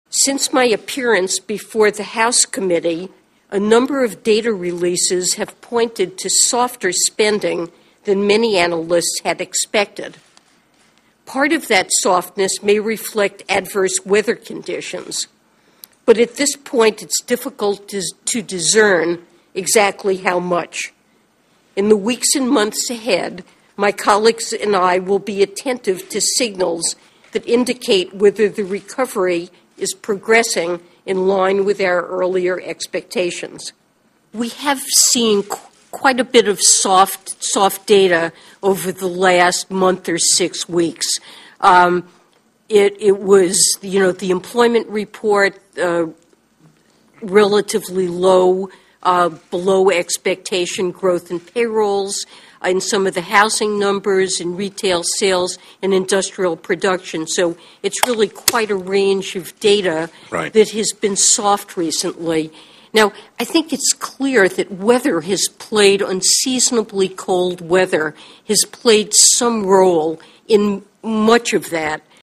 Yellen: Fed Monitors Recent Weaker Economic Data
Federal Reserve Chair Janet Yellen noted Thursday that some recent economic data have pointed to weaker-than-expected gains in consumer spending and job growth. The Fed will watch to see whether the slowdown was caused by severe winter weather. (Feb. 27)